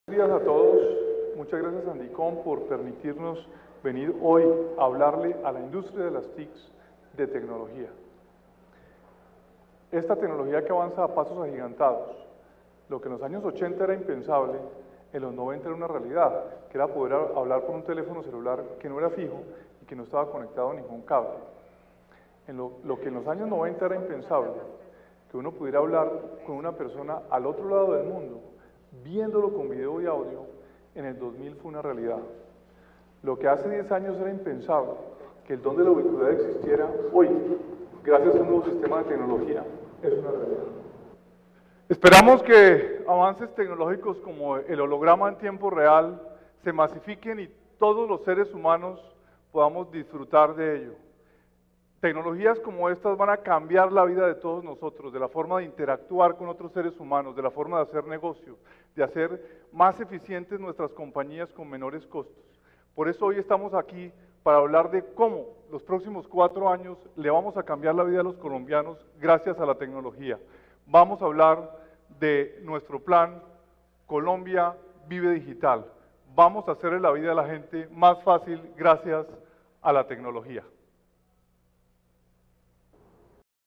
Presentación de Vive Digital, Diego Molano Vega Ministro TIC
El Ministro hace la  introducción para la presentación del Plan Vive Digital.